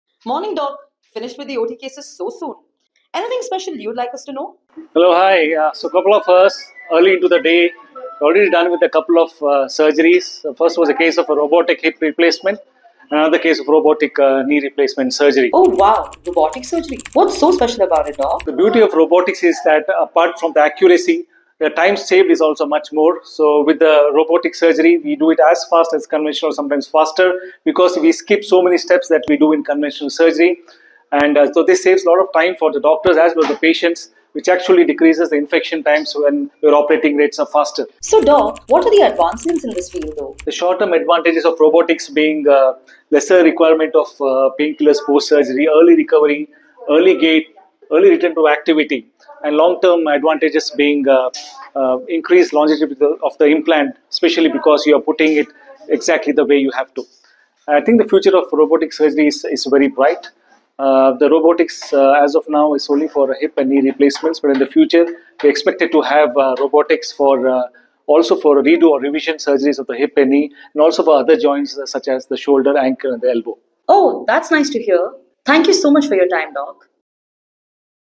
Know More About Robotic Surgeries | Dr. Sunil G Kini | MHB
Robotic surgery is revolutionizing joint replacements with precision, speed, and better outcomes.

In this video, our expert surgeon shares how robotic technology is making hip and knee replacements faster, more accurate, and less painful. It means quicker recovery, fewer infections, and longer-lasting implants. And this is just the start; soon, we’ll see robotics used for other joints like shoulders, ankles, and elbows.

Studies show that robotic-assisted surgeries can reduce recovery time by a margin. A significant leap forward in patient care is here.